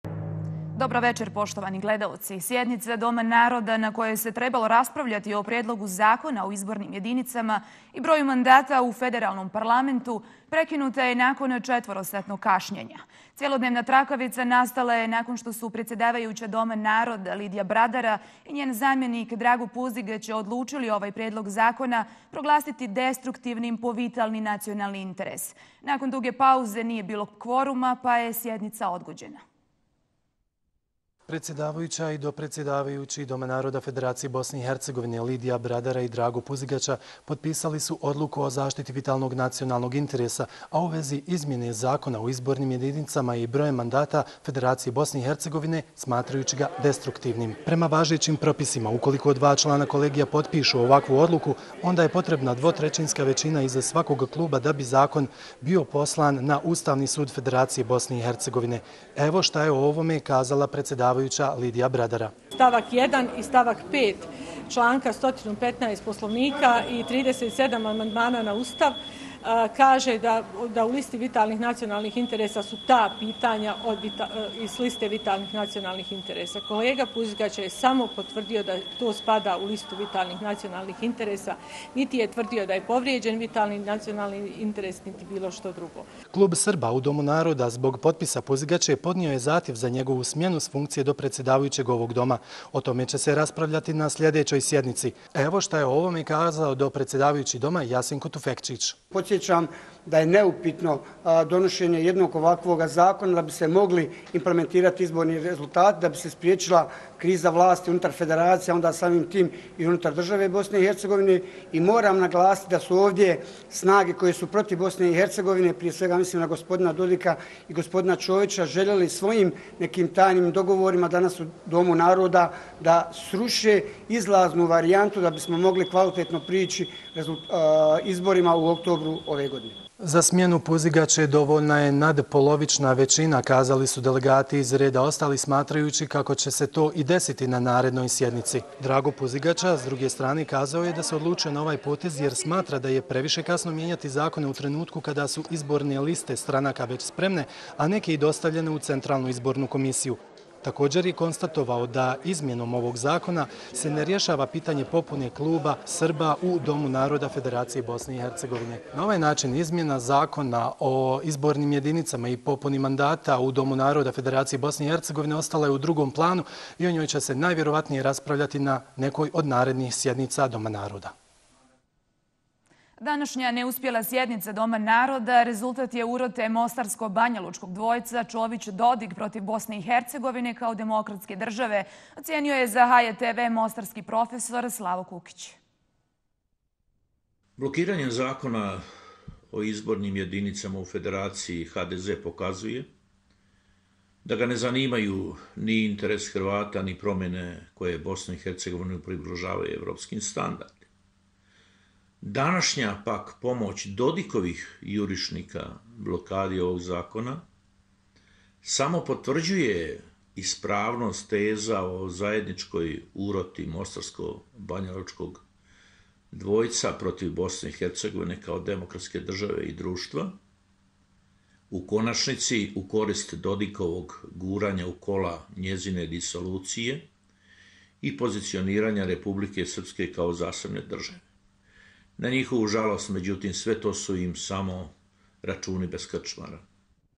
SRBI ŠTITE VITALNI NACIONALNI INTERES HRVATA, NIŠTA OD IZMJENA ZAKONA O IZBORNIM JEDINICAMA!
Sjednica doma naroda, na kojoj se trebalo raspravljati o Prijedlogu zakona o izbornim jedinicima i broju mandata u Federalnom parlamentu, prekinuta je nakon četverostanog kašnjenja. Cjelodnevna trakavica nastala je nakon što su predsjedavajuća Doma naroda Lidija Bradara i njen zamjenik Drago Puzigaća odlučili ovaj prijedlog zakona proglasiti destruktivnim po vitalni nacionalni interes. Nakon duge pauze nije bilo kvoruma, pa je sjednica odgođena.

"Današnja neuspjela sjednica Doma naroda rezultat je urote mostarsko-banjalučkog dvojca Čović-Dodik protiv BiH kao demokratske države", ocijenio je za Hayat TV mostarski profesor Slavo Kukić.

(Emerin Ahmetašević/ Dženeta Sarić – Kozić/Hayat/Foto:Hayat)